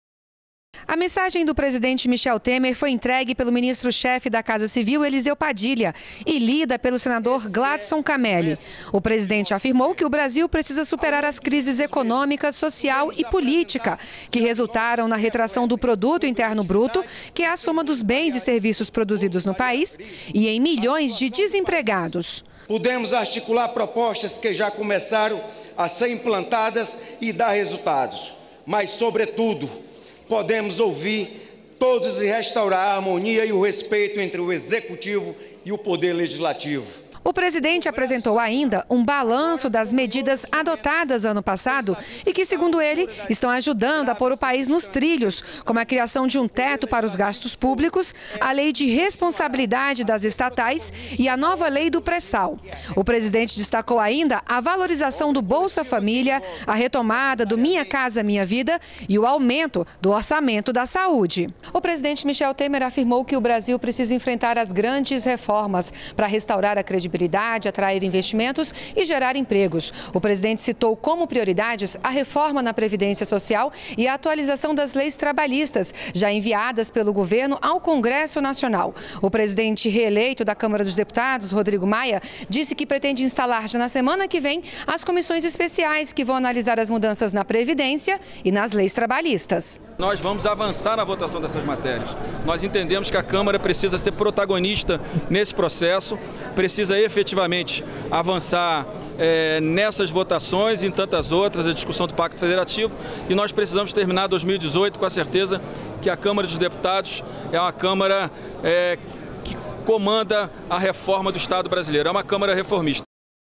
Em mensagem ao Congresso, Temer defende reformas Trabalhista e da Previdência
REPÓRTER NBR - 03.02.17: Presidente Michel Temer enviou nesta quinta-feira (2) mensagem ao Congresso Nacional por ocasião da abertura dos trabalhos legislativos de 2017. Na carta, Temer afirmou que o Brasil precisa superar as crises econômicas, sociais e políticas e defendeu a aprovação de reformas, como a Trabalhista e a da Previdência.